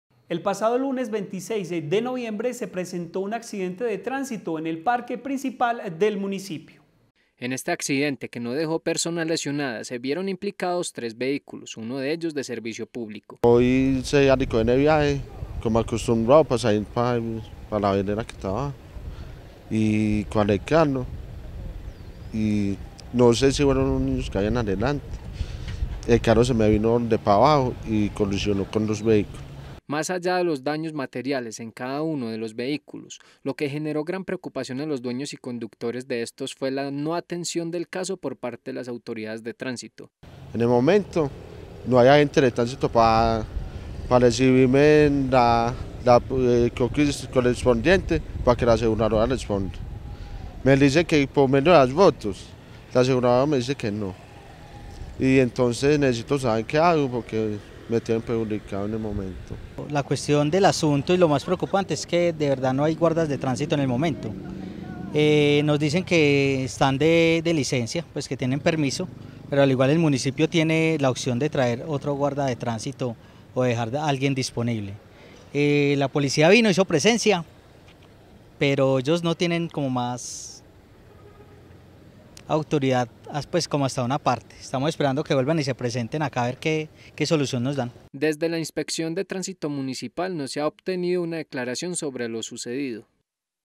ACCIDENTE SIN ATENCIÓN DEL TRÁNSITO
El pasado lunes 26 de noviembre se presentó un accidente de tránsito en el parque principal del municipio.